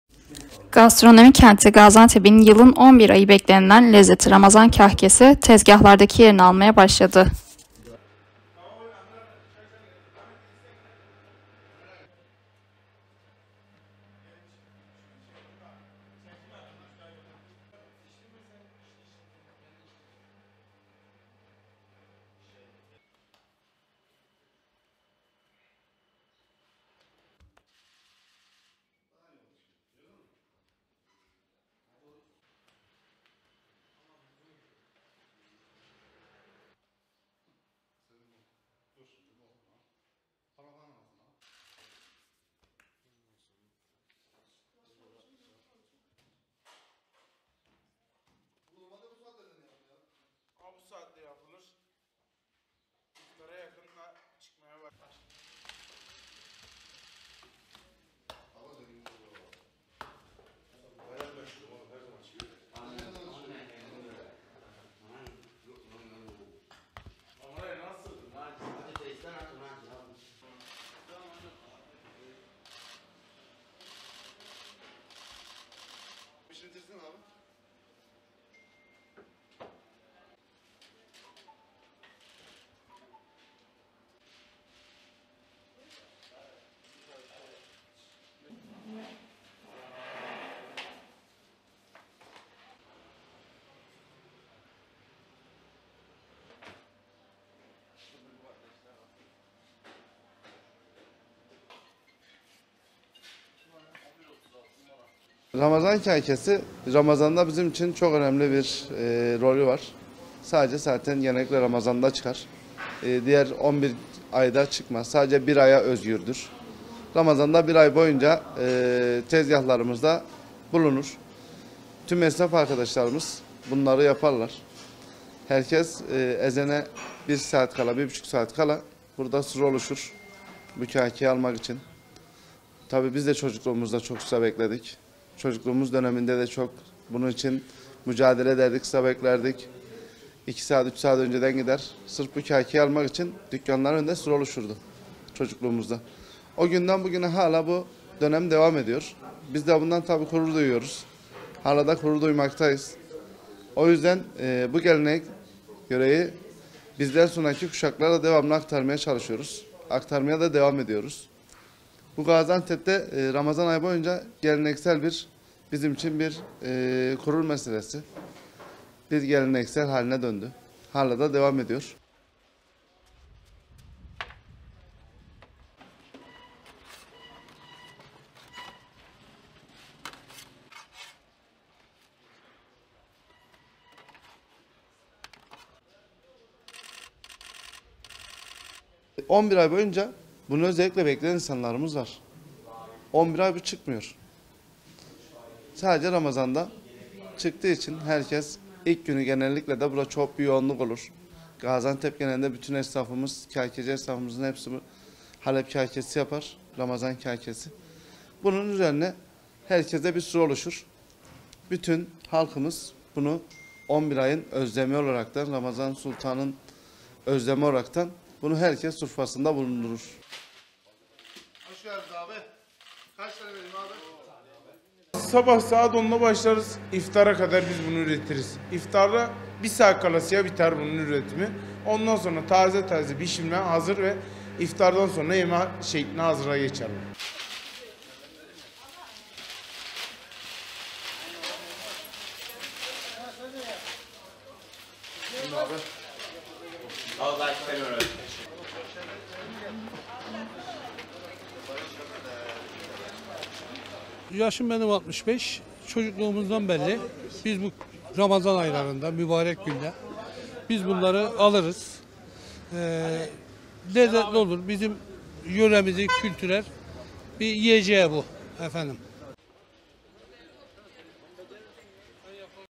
11 Ay Beklenen Lezzet: Ramazan Kahkesi
Gaziantep'te ramazan ayına özel olarak üretilen ve bölge halkı tarafından 11 ay beklenilen ramazan kahkesi, tezgahlarda ve sofralardaki yerini almaya başladı